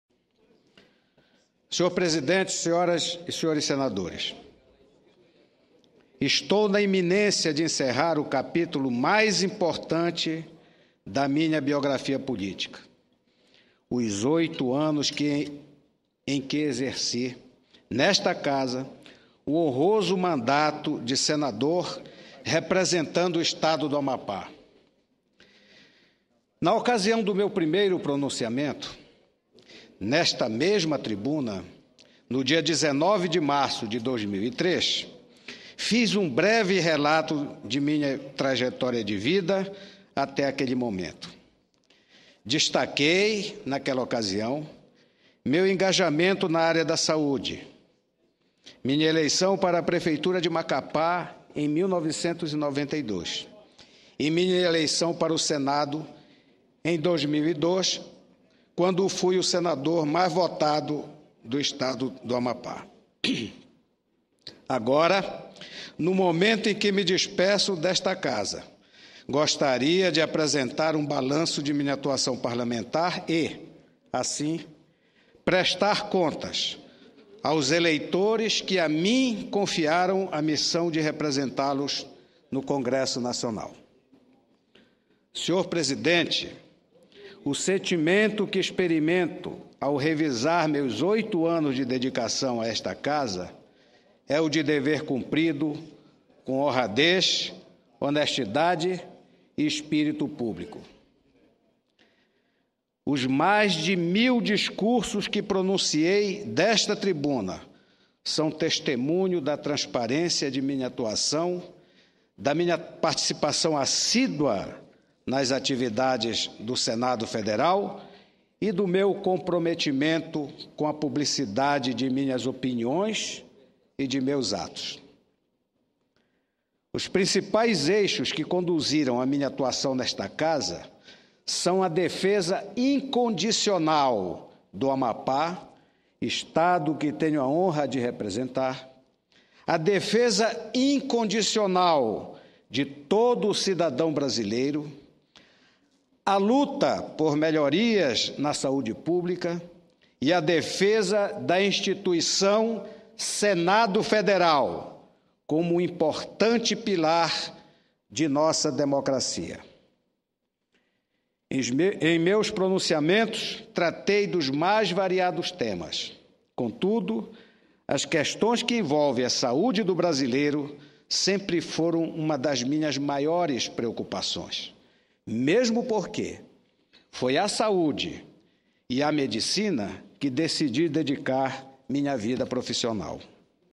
Sen. Papaléo Paes, PSDB/AP, anuncia balanço de suas atividades como senador
14/12/2010 - Plenário - Sen. Papaléo Paes, PSDB/AP, anuncia balanço de suas atividades como senador